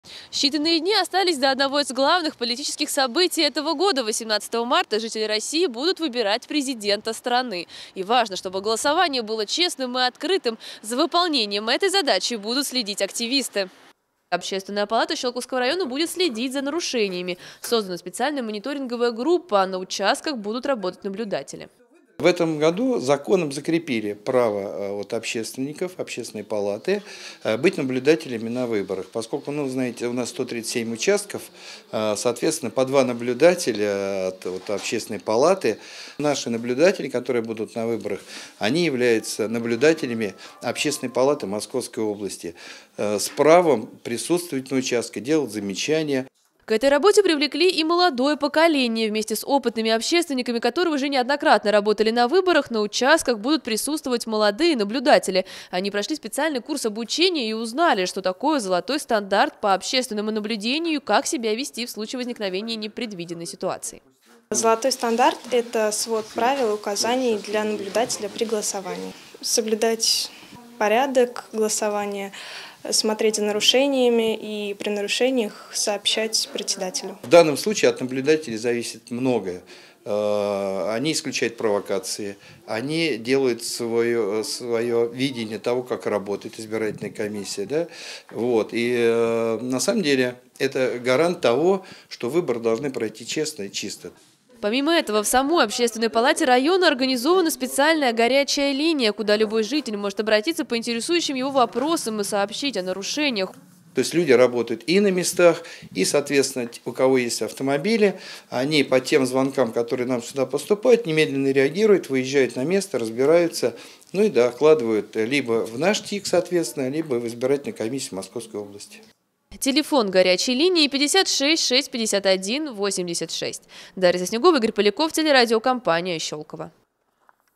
За выборами Президента России будут следить наблюдатели от Общественной палаты
Честно и прозрачно. В выборах Президента России примут участие наблюдатели от Общественной палаты. Они будут присутствовать на участках и следить за тем, чтобы голосование проходило без нарушений. Подробнее расскажем далее.